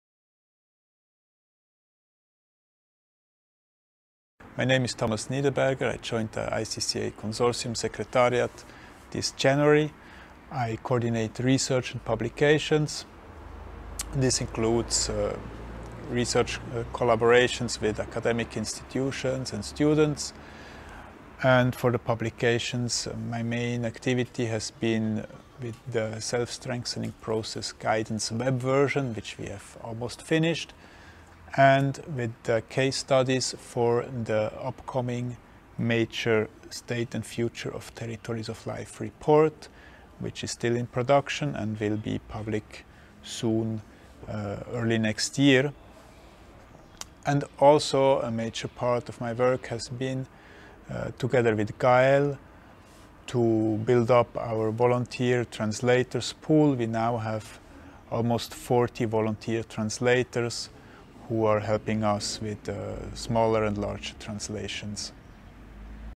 Research and Publications | Global Report | 16th General Assembly | ICCA Consortium
Presented by Thomas Niederberger (Research and Publications Coordinator), as part of the 2019/2020 programme update during the ICCA Consortium's 16th General Assembly (28-30 October 2020).

Presentado por Thomas Niederberger (Coordinador de Investigación y Publicaciones), como parte de la actualización del programa 2019/2020 durante la 16ª Asamblea General del Consorcio TICCA (28-30 de octubre de 2020).

Présenté par Thomas Niederberger (Coordinateur de la recherche et des publications), dans le cadre de la mise à jour du programme 2019/2020 lors de la 16e Assemblée générale du Consortium APAC (28-30 octobre 2020).